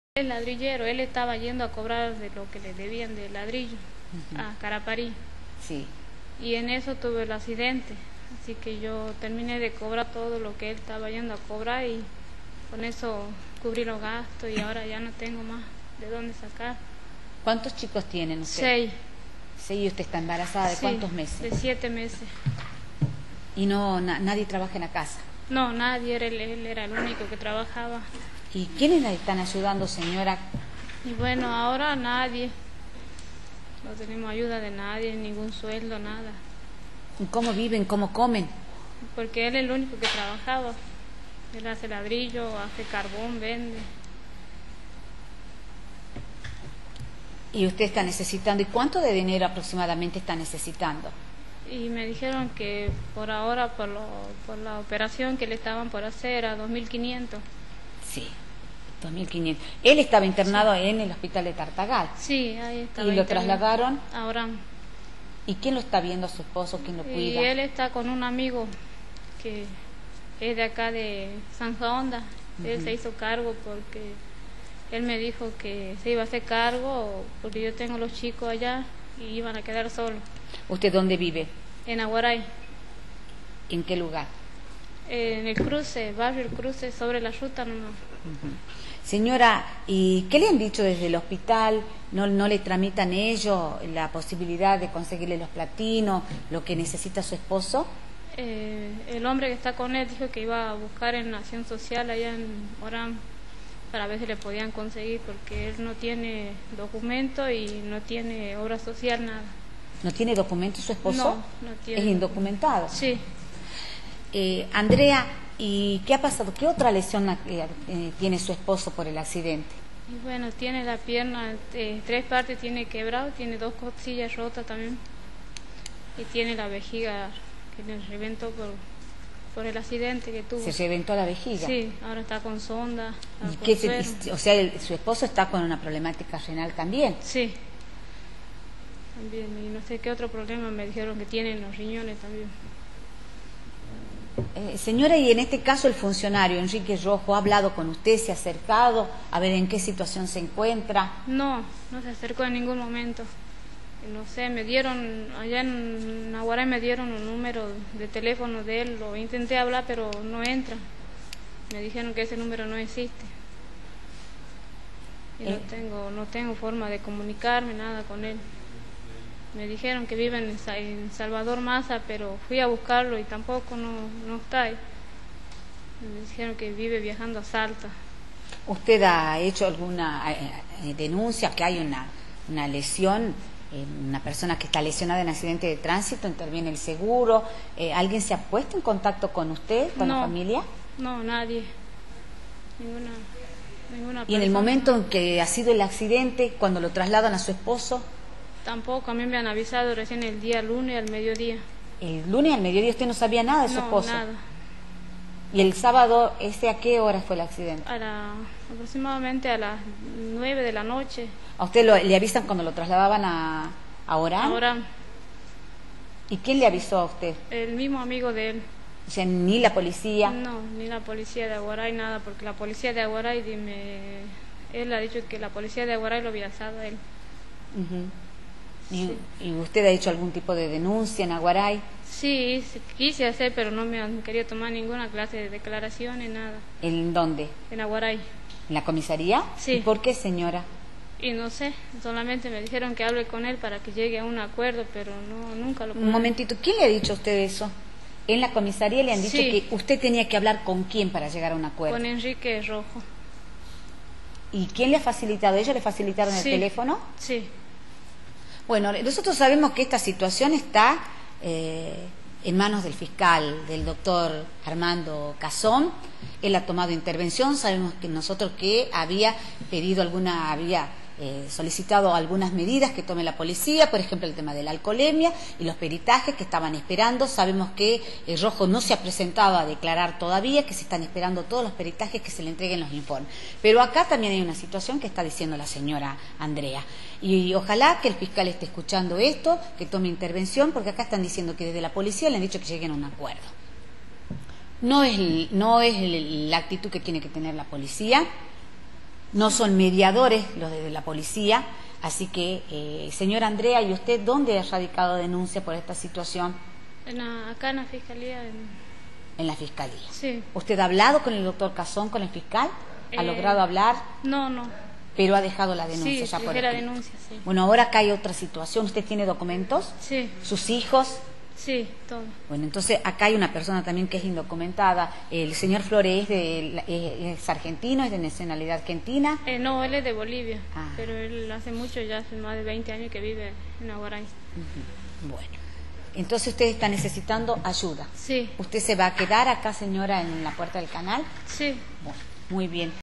Transito en rojo
El accidente de tránsito protagonizado por Enrique Rojo, Subsecretario de Pueblos Originarios, dejo  a Luis Flores, un obrero de la construcción de Aguaray con varias fracturas internado en el hospital. La esposa del albañil, embarazada de 7 meses, pidió ayuda desesperada para sus 6 hijos ya que es el que mantiene a la familia. Andrea Díaz dijo que Rojo jamás se acercó a preguntar por la salud del herido y también denuncio que la denuncia la realizo en la fiscalía ya que la policía de su localidad nunca le quiso recibir el escrito.